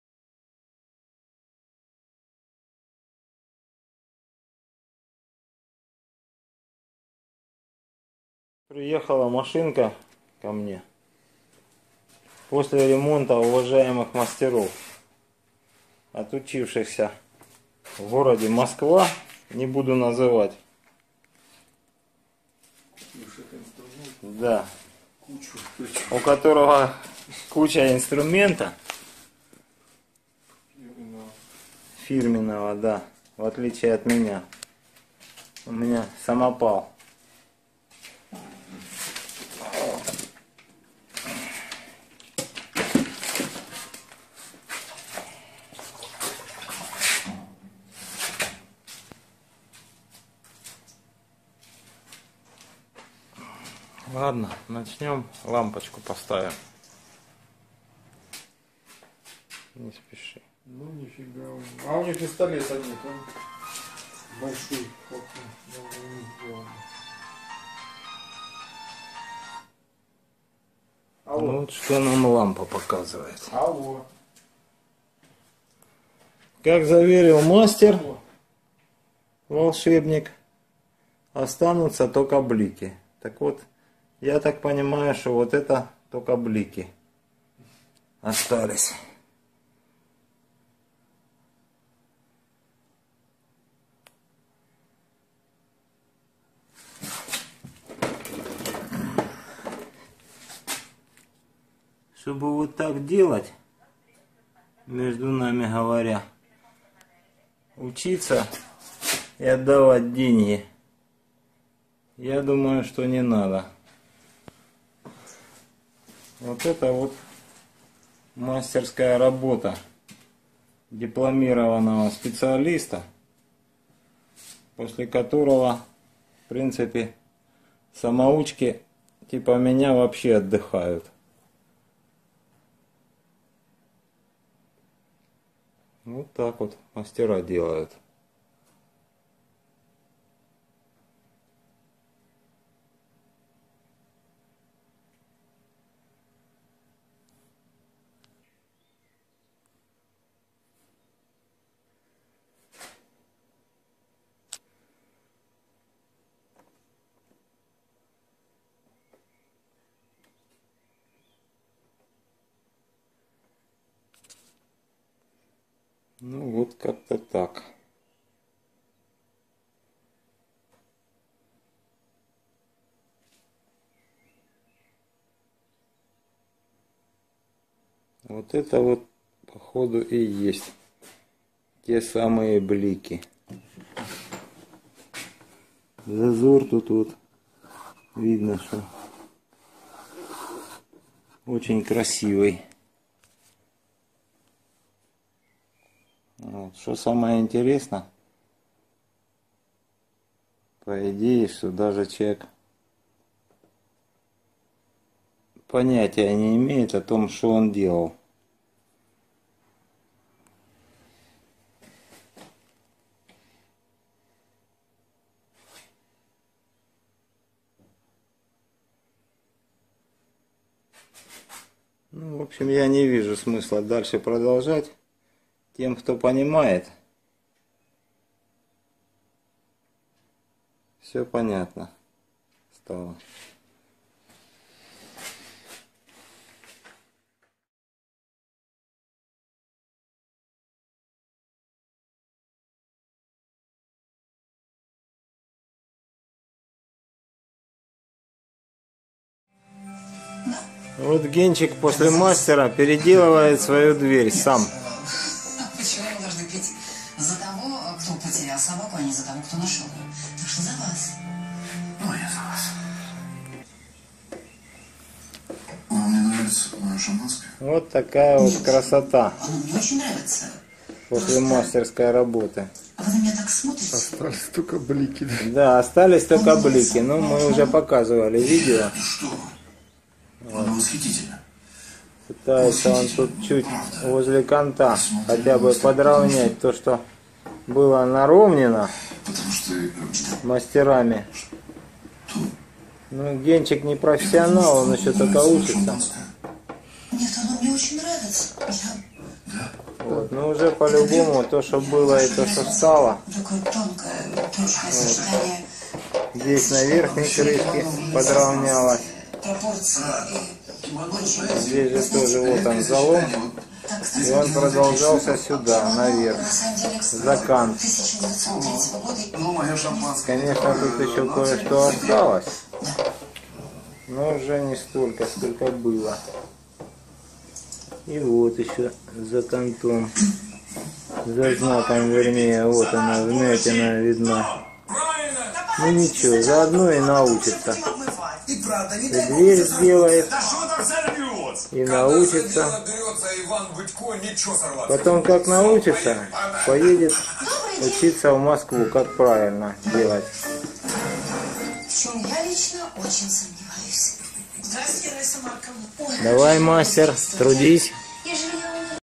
alteration after the wizard PDR - переделка после мастера PDR
прежде  чем  брать деталь в  ремонт  надо  не  дипломом  махать  а подумать  по силам  ли   отремонтировать  данное повреждение